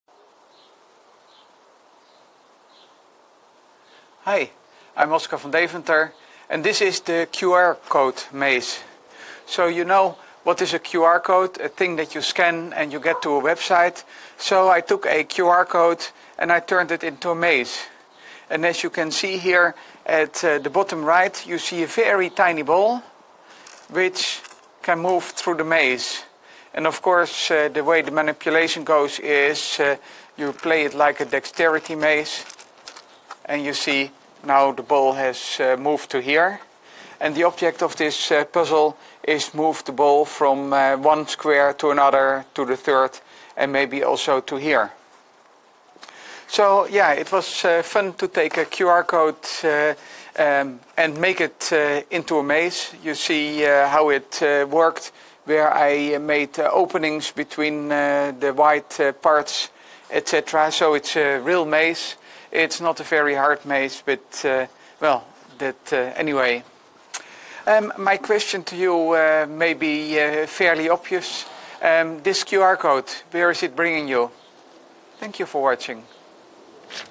QR-Code Maze - Whereto does this QR code lead you?
QR-Code Maze is a self-referential maze. The object is to roll the 3-mm ball through the maze from square to square. The pattern of the maze is a QR code. When you scan the QR code, you get the hyperlink which is where you can download the files for the QR-Code Maze and print one for yourself.